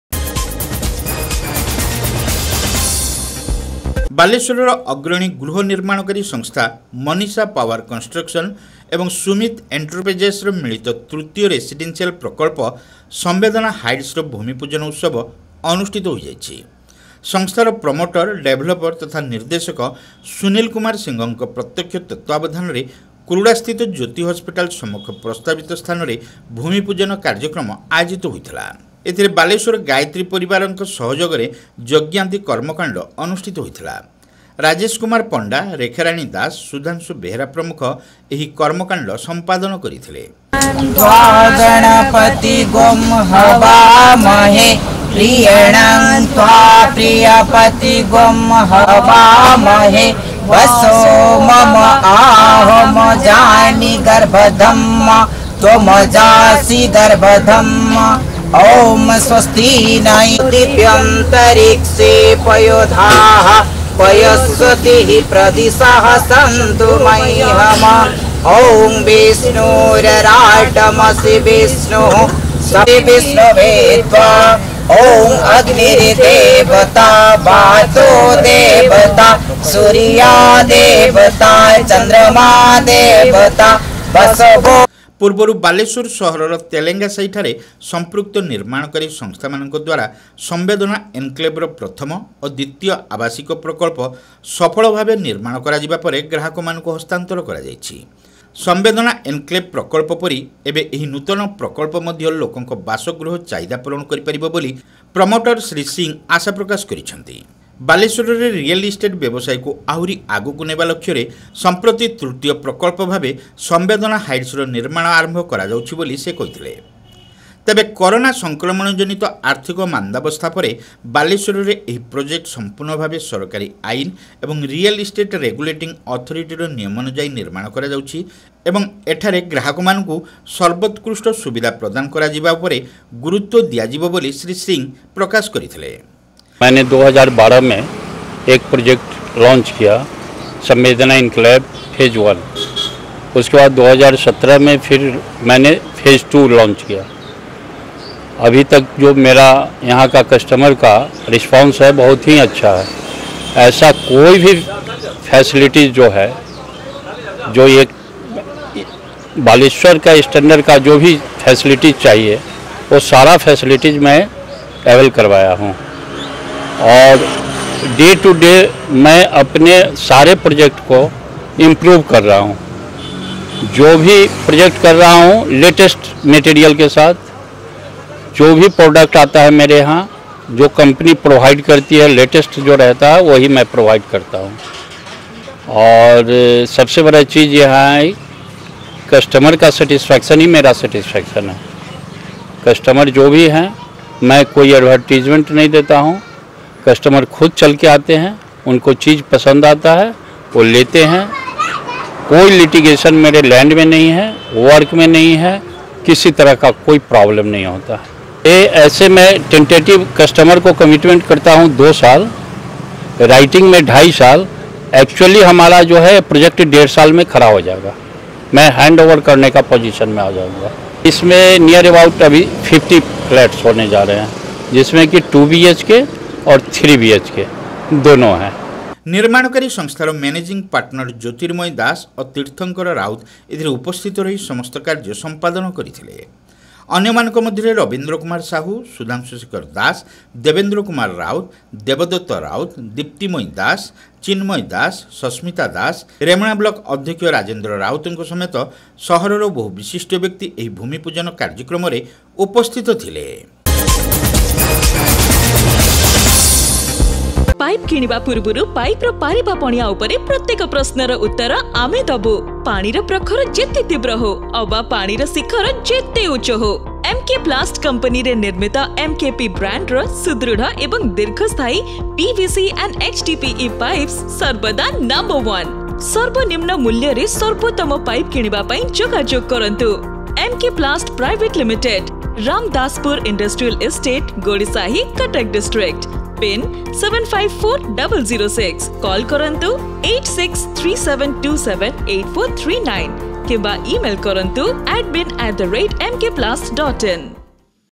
ମାଧବ ନ୍ୟୁଜ - ବାଲେଶ୍ଵରରେ ଅନୁଷ୍ଠିତ ହେଲା ନୂତନ ହାଉସିଂ ପ୍ରୋଜେକ୍ଟ ସମ୍ବେଦନା ହାଇଟ୍ସର ଭୂମିପୂଜନ
Madhab News - Bhoomi Pujan for Samedana Heights Residential Project Held at Balasore - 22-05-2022